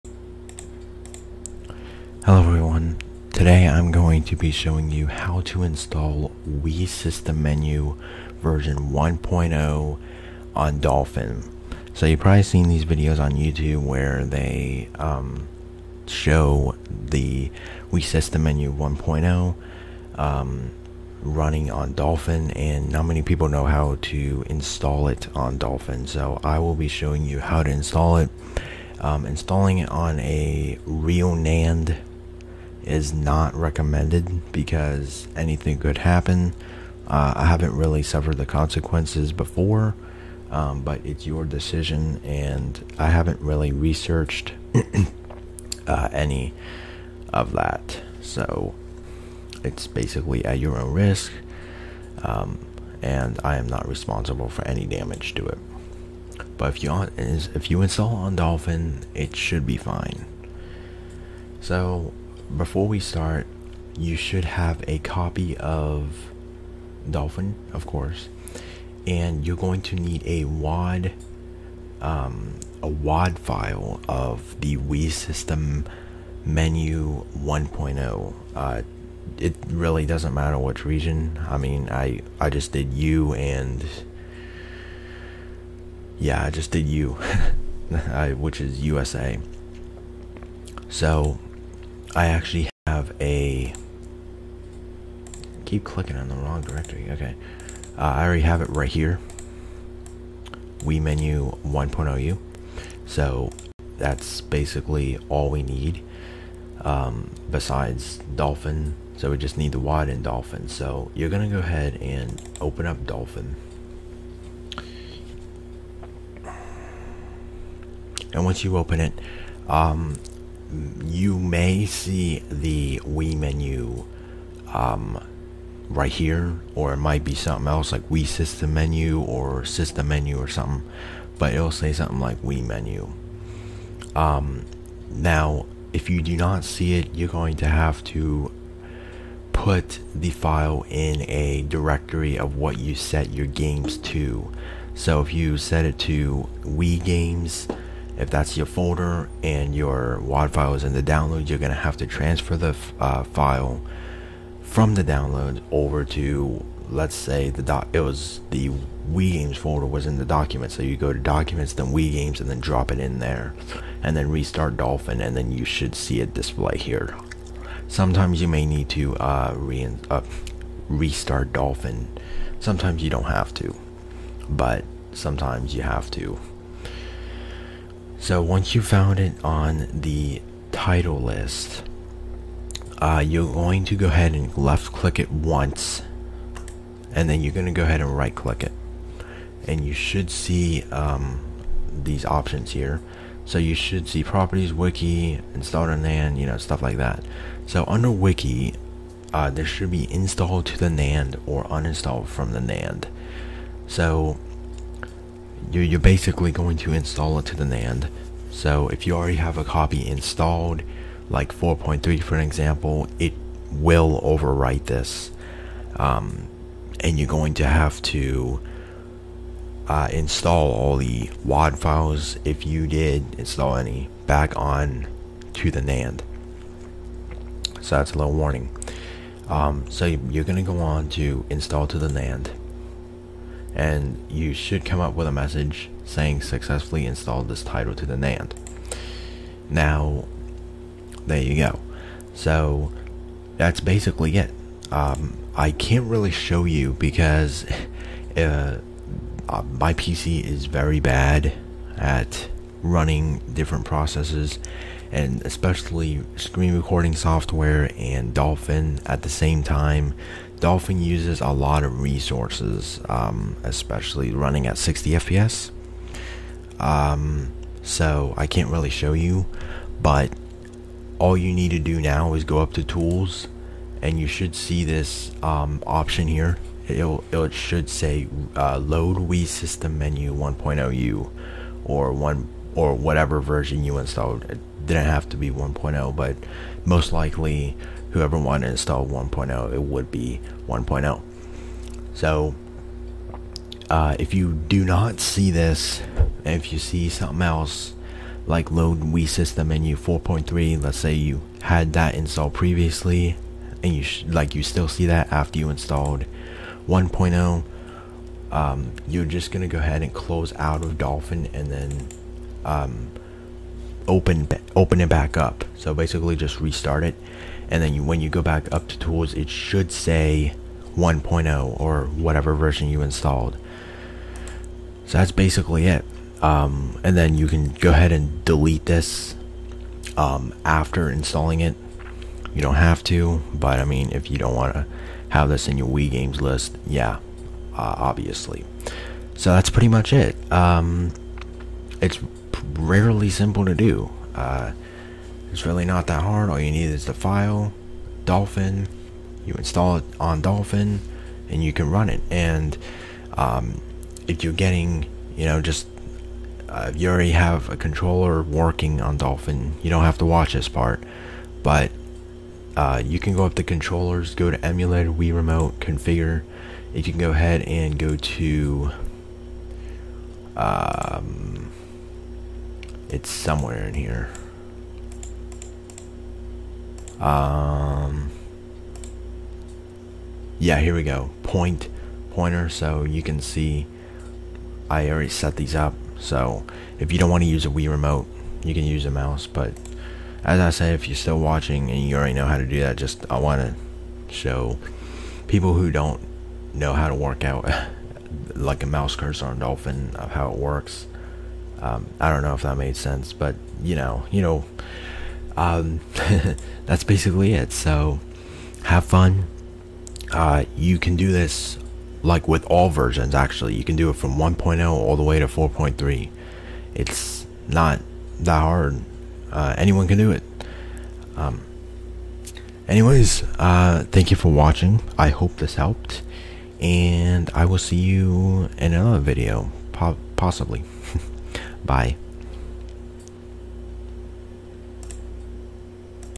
How to install Wii System Menu v1.0 onto Dolphin
In today's video, I will be going over how to install v1.0 (or any version) Wii System Menu onto Dolphin.

I apologize for any audio cut-offs or lag issues. My PC cannot handle much, I can tell you that.

If you encounter any issues following the tutorial, be sure to notify me in the comments, and I will do my best to help you out.